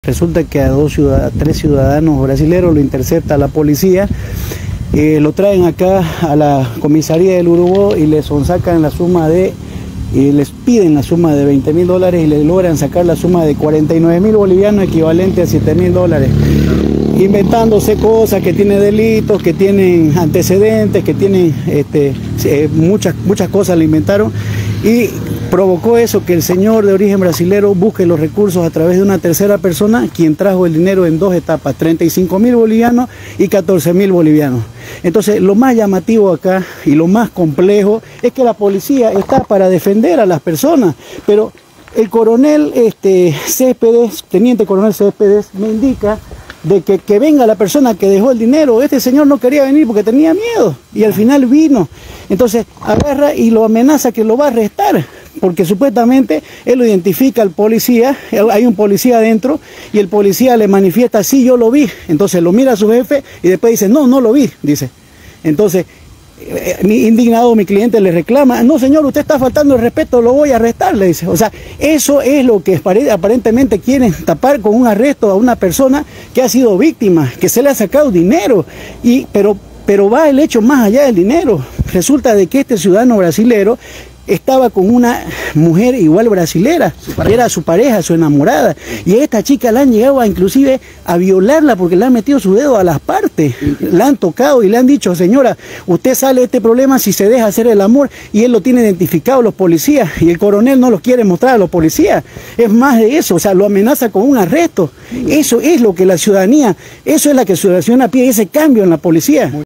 BRASILEROS DENUNCIAN A POLICÍAS DE LA COMISARÍA DEL URUBÓ POR VARIOS DELITOS
La defensa legal de 4 brasileños denunció que policías de la comisaria del Urubó detuvieron a sus clientes   y los llevaron a las oficinas con el pretexto de investigar supuestos antecedentes criminales.
En el lugar los oficiales de turno les pidieron 20.000 dólares para dejarlos libres y habrían abusado de una joven modelo.